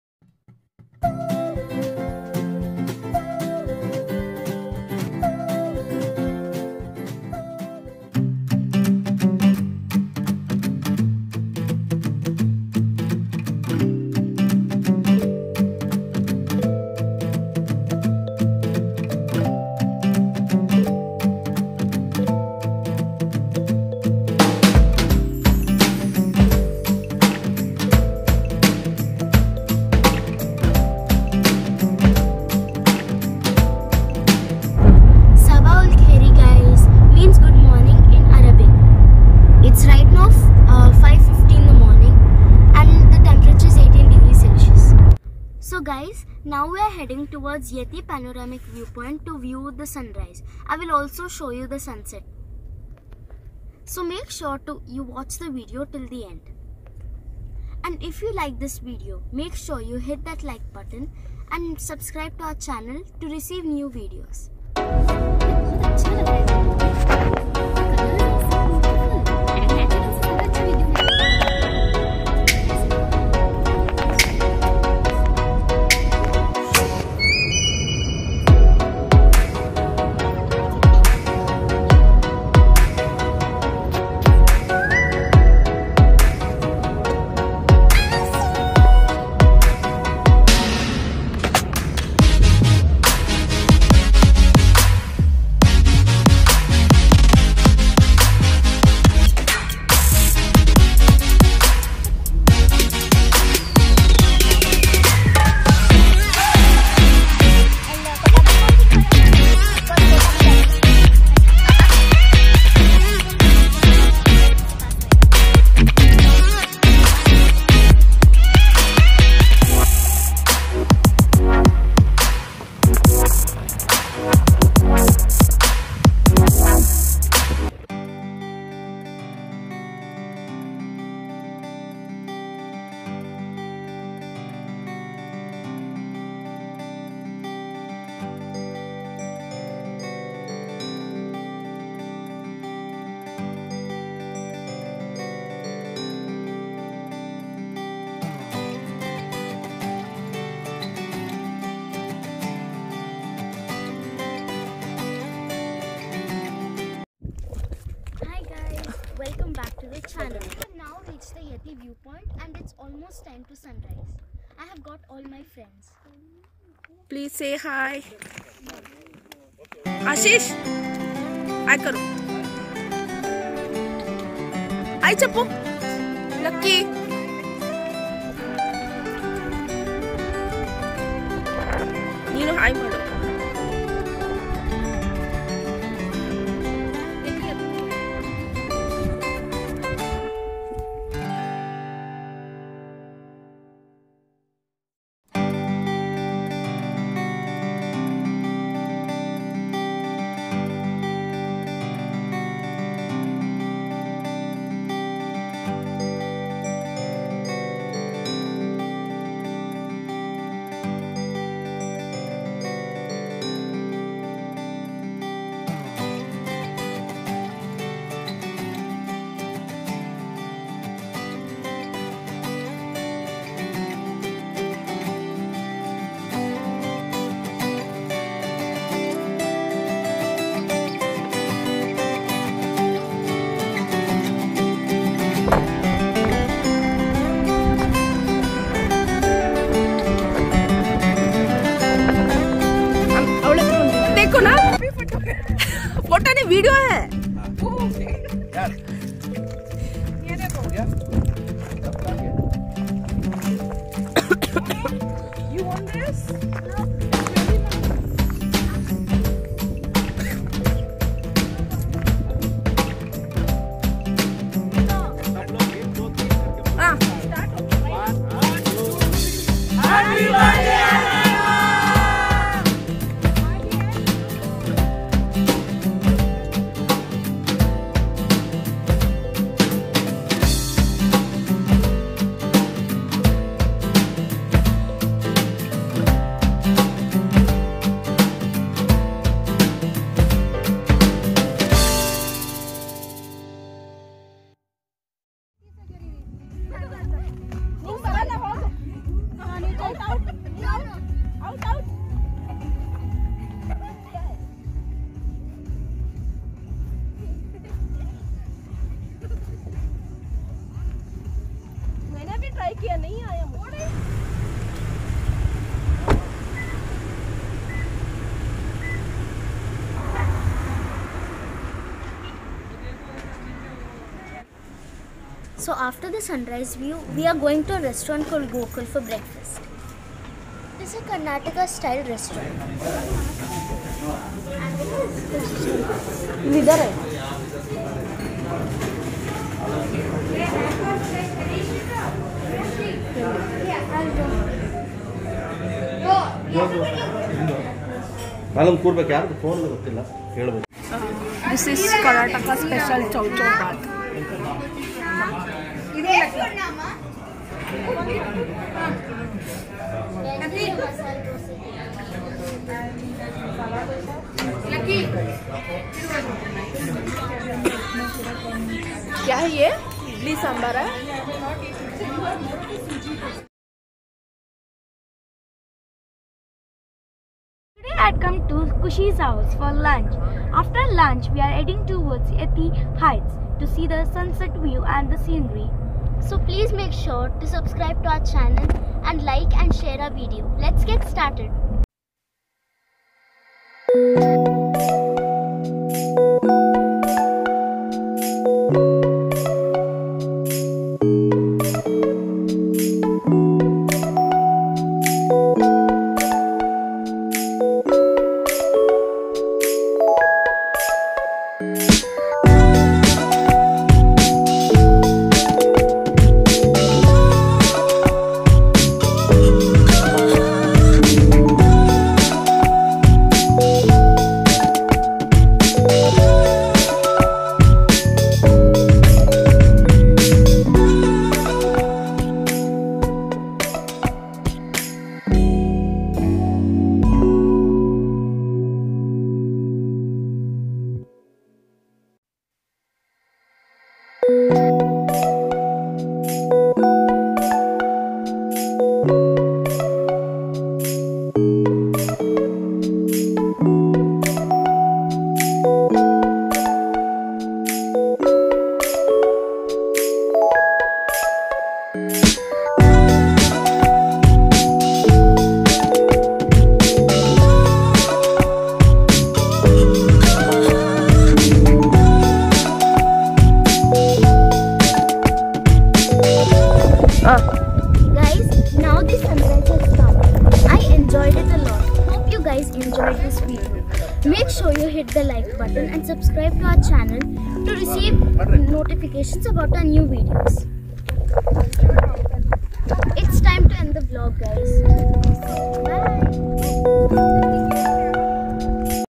YITI Panaromic view of Sunrise 🌅 Sunset in Muscat
YITI Panaromic view point of both sunrise and Sunset in Muscat
we had good Friday breakfast in  Gokul restaurant located in Wadi kabir